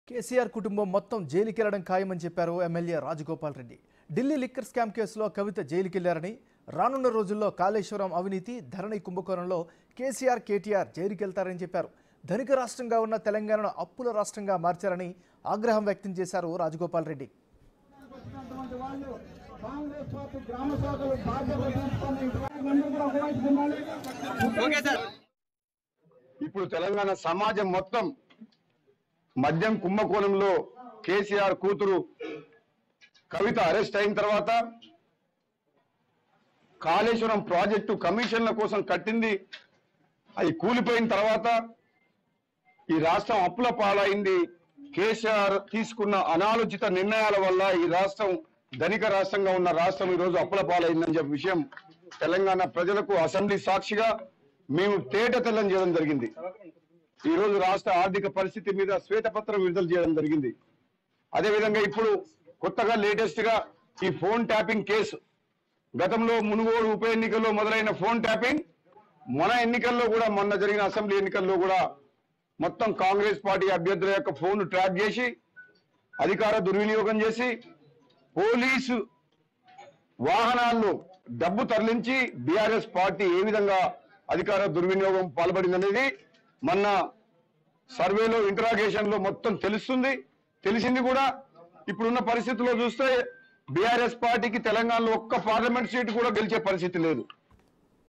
Raj Gopal Reddy Sensational Comments On KCR & KTR | కేసీఆర్,కేటీఆర్ కు జైలే గతి | BIG TV
MLA Rajagopal Reddy said that KCR's entire family will be jailed. Kavitha was jailed in the Delhi liquor scam case.
In the coming days, KCR and KTR will go to jail in Kaleshwaram corruption and Dharani scandal. Rajagopal Reddy expressed anger that Telangana, which was a rich state, was turned into a debt state.